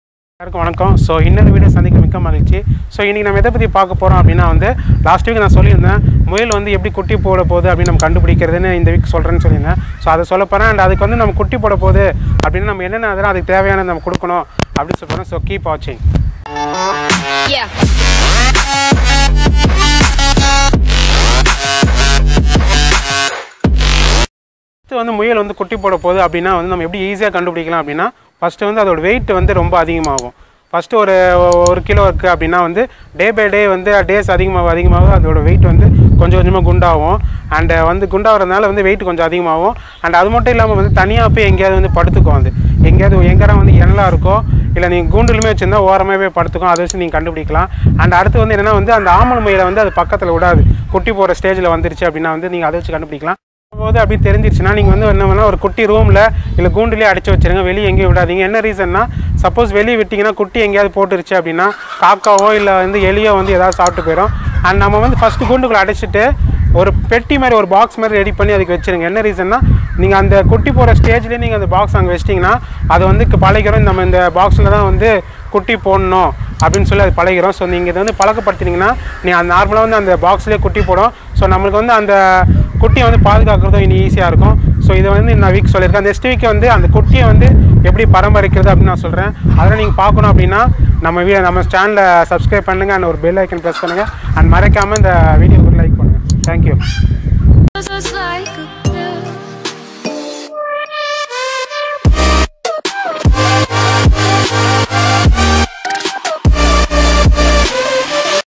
How to find Your rabbit is Pregnant ?? Full Explanation in Tamil
Hi Guys , Thanks for all your Support till time,

Expecting Same Support for this Video, Today In this Video , I am going to Speak about

"How to find our rabbit is pregnant?

My Another Channel  for Weight Loss Tips -

The Success Story Behind Successful Egg Hatching !!!